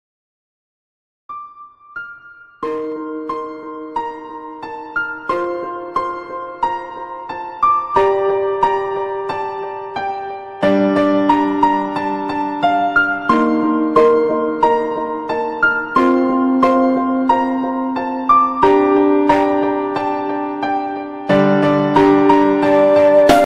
ANTONOV AN225.🥹🥹🫶🏻❤️..RIP..
Antonov AN225, it was designed and built throughout the 1980s and took its maiden flight on 21st December 1988. A second AN-225 began production in the late 1980s but, in 2009 when the aircraft had still not yet been completed, production was abandoned. The AN-225 was destroyed in February 2022.🥹

pls support guys❤️❤️🫶🏻